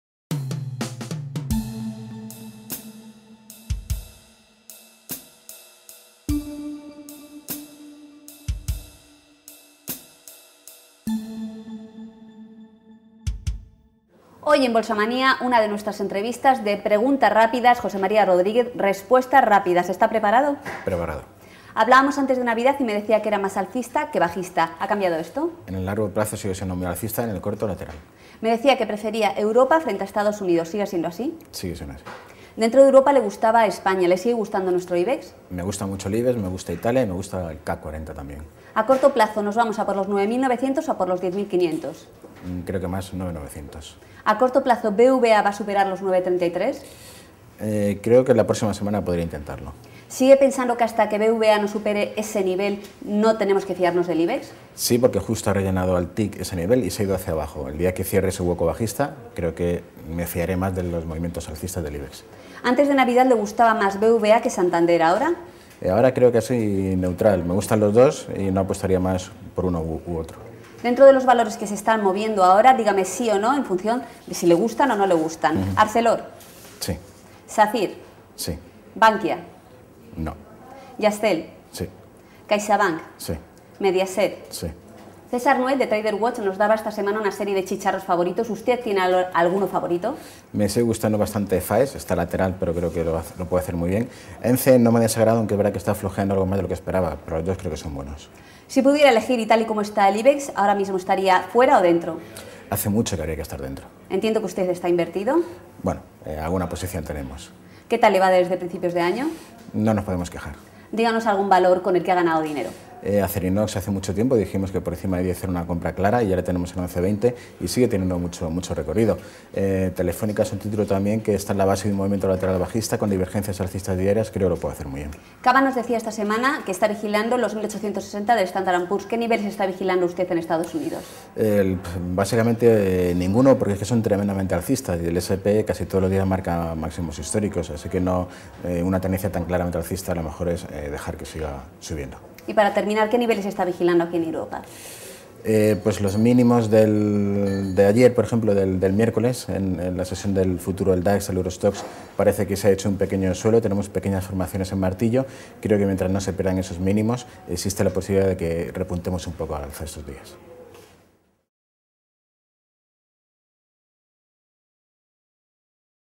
De primera mano
A largo plazo, José María Rodríguez, experto de Bolsamanía, sigue siendo "muy alcista". Sin embargo, a corto, no tanto. Así resume las dudas que ahora dominan a un Ibex que es más fácil que se vaya a por los 9.900 en las próximas semanas que a por los 10.500. Eso sí, mucho de ello dependerá de lo que haga BBVA con ese nivel de los 9,33. En cuanto a la entidad vasca, le gusta tanto como Santander y, dentro de los "chicharros", apuesta por Faes.